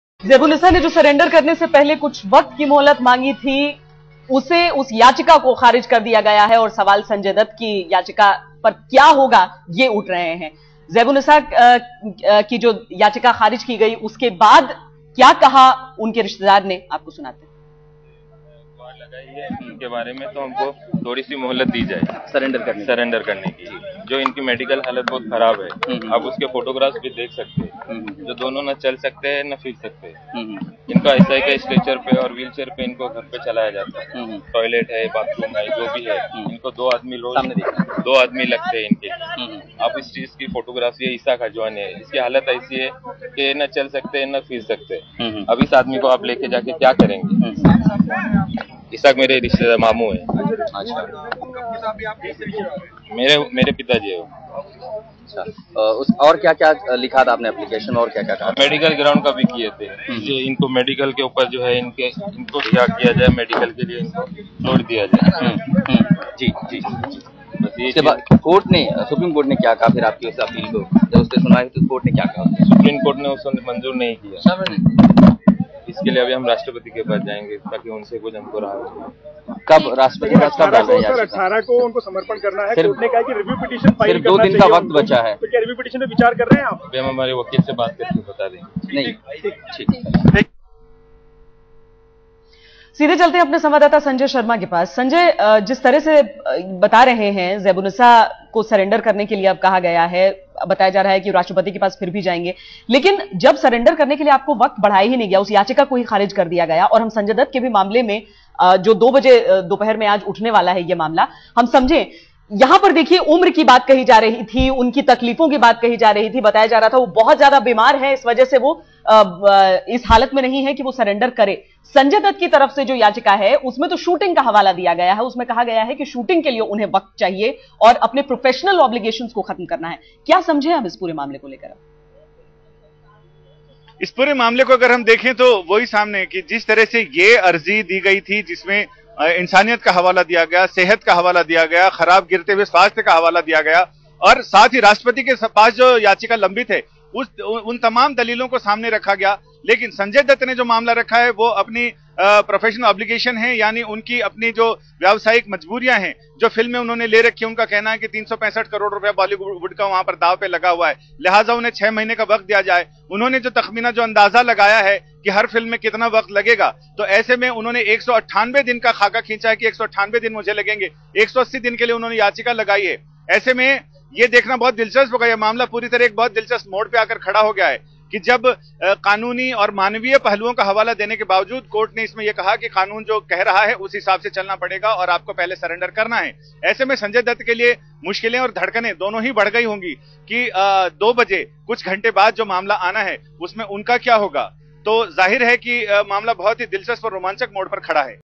SC dismisses plea of Zaibunisa seeking more time to surrender
The Supreme Court on Tuesday dismissed the plea by 1993 Mumbai serial blast case convicts Zaibunisa and two others seeking extension of time to surrender to serve remaing jail term.